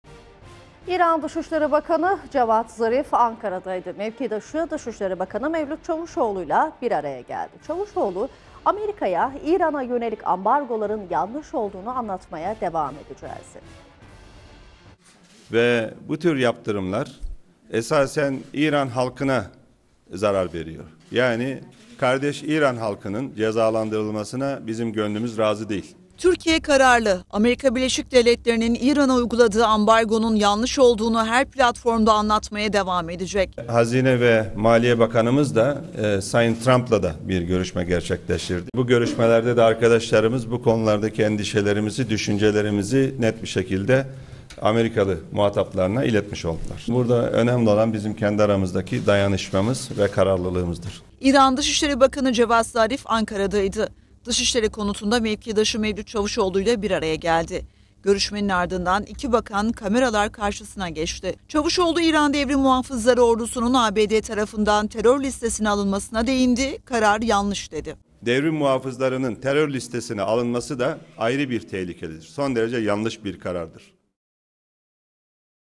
Bakan Çavuşoğlu: Yanlışlığı Trump'a İlettik
İran Dışişleri Bakanı Cevad Zarif, Ankara'daydı. Mevkidaşı Dışişleri Bakanı Mevlüt Çavuşoğlu ile bir araya geldi. Çavuşoğlu, "ABD'ye İran'a yönelik ambargoların yanlış olduğunu devam edeceğiz" dedi.